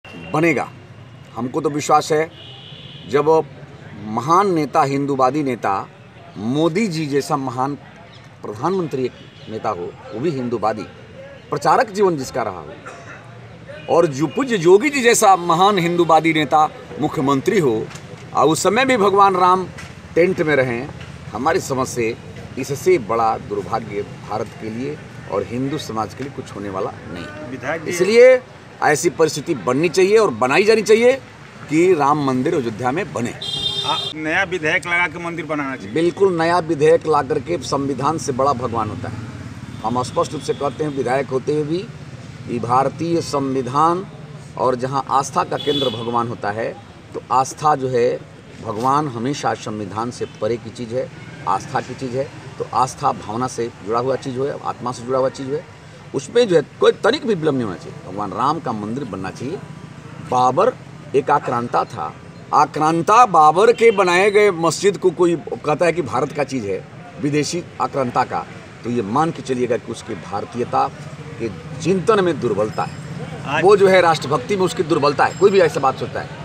बलियाः मोदी-योगी जैसे नेता के रहते भगवान राम को टेंट में रहना दुर्भाग्यपूर्ण- सुरेंद्र सिंह
बलिया: भाजपा विधायक सुरेंद्र सिंह एक बार फिर राम मंदिर के मुद्दे पर अपने ही सरकार को घेरा है. उन्होंने कहा कि केद्र और राज्य में मोदी और योगी की सरकार है, ऐसे में भगवान राम को टेंट में रहना दुर्भाग्यपूर्ण है.